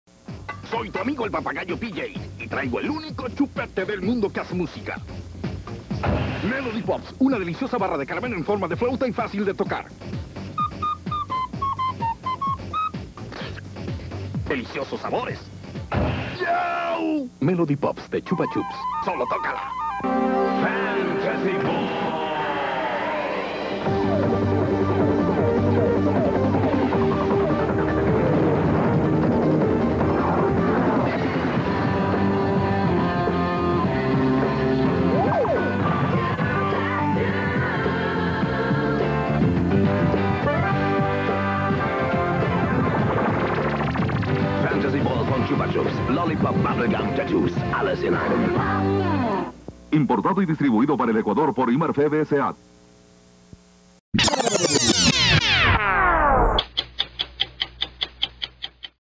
Comerciales CHUPA CHUPS - MELODY POPS - LOLLI POP - Comerciales antiguos ecuatorianos - noventero
Comercial de los chupetes MELODY POPS y LOLLI POP de CHUPA CHUPS.  Este comercial fue transmitido por TC Televisión (Guayaquil - Ecuador) en diciembre de 1997.  El video fue conservado en un tape de VHS, recuperado y digitalizado por Cronóstatos en 2022.

Melody Pops esa una barra de caramelo con forma de flauta, cuyas tonalidades podían ser alteradas desplazando manualmente un émbolo longitudinal que se encontraba acoplado al interior de la barra. En este comercial aparece el papagayo PJ.

Los productos de CHUPA CHUPS era importados y distribuidos en Ecuador por IMARFEVE S.A., de Guayaquil.